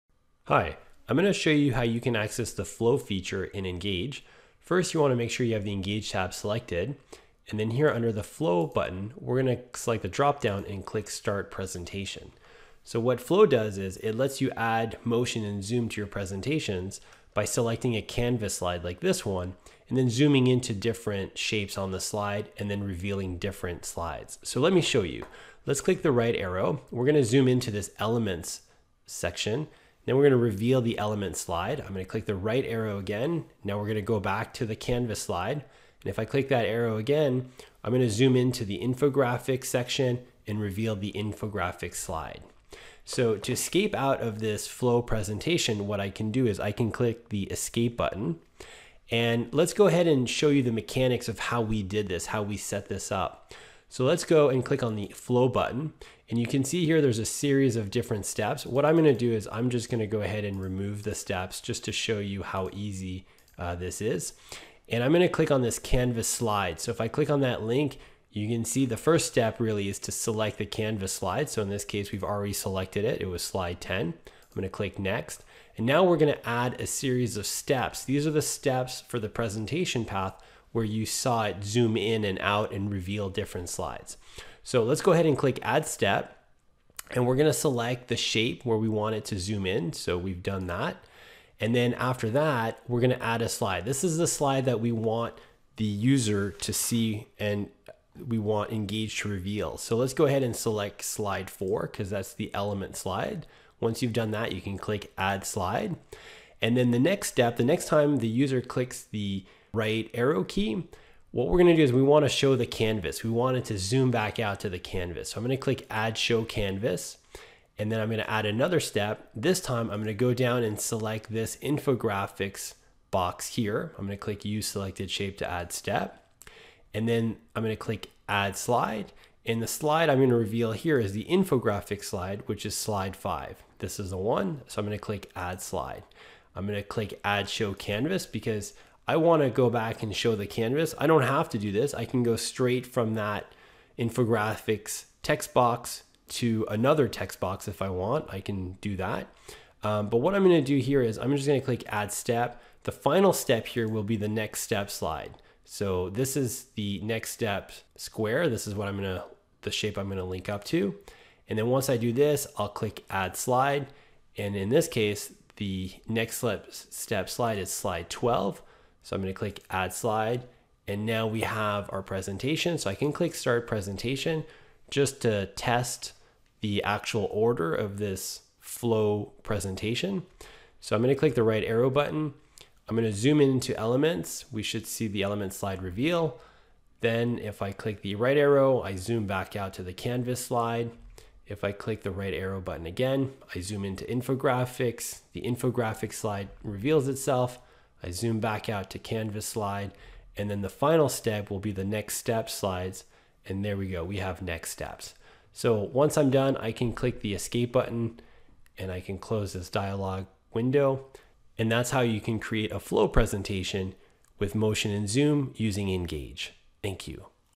Flow - Getting Started with the Engage PowerPoint add-in
Add motion and zoom to any presentation

Turn any presentation into an attention grabbing presentation by adding motion and zoom right in PowerPoint.